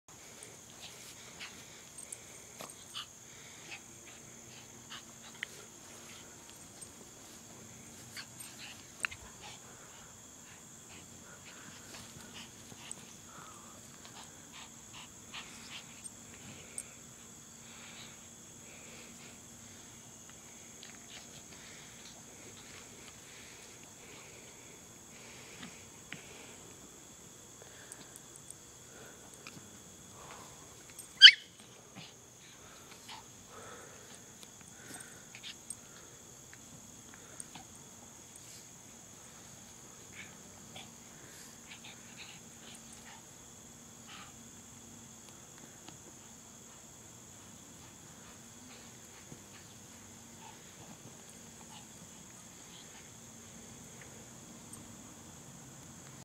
Japanese Chins Playing
I have two Japanese Chins who love to play. Enjoy 🐶😌